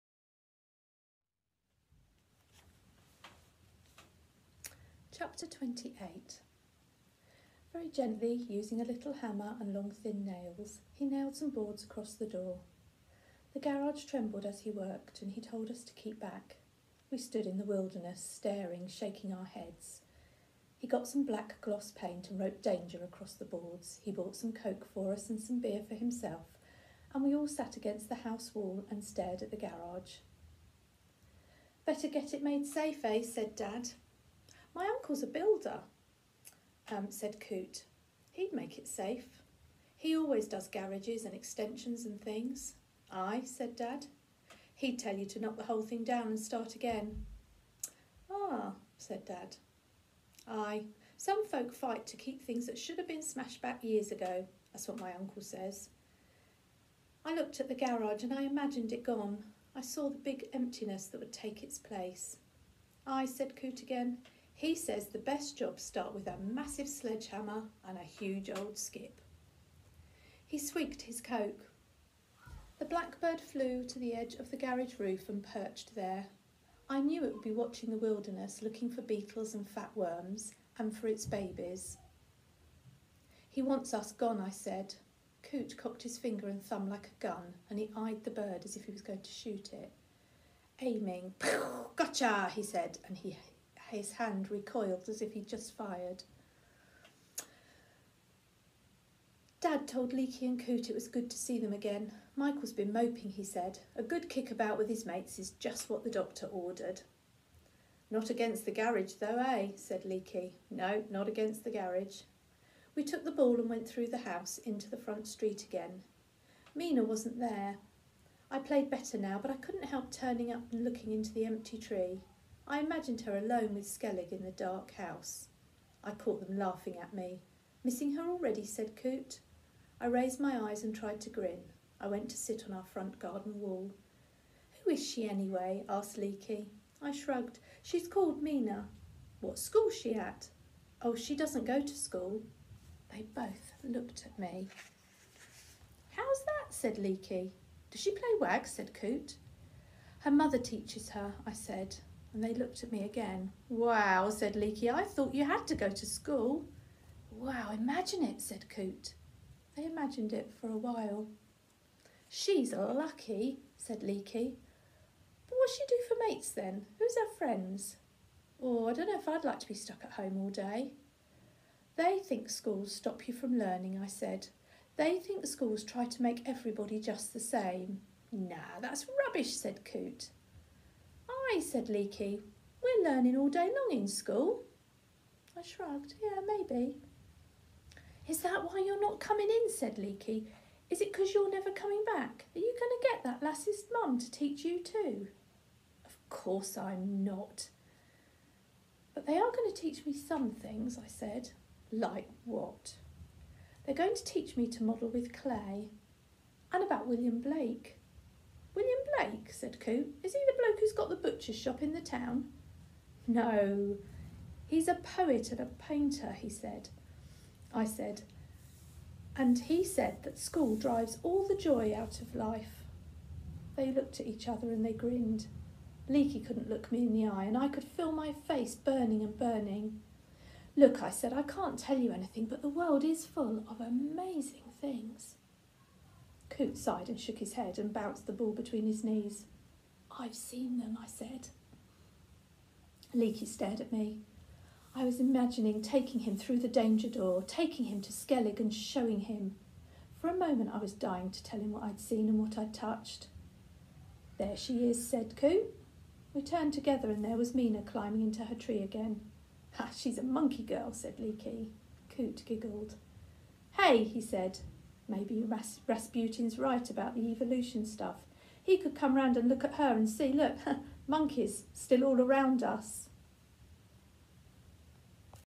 Year 5 English - Skellig, Chapter 28
Mrs Berbiers reads Chapter 28 of Skellig, by David Almond.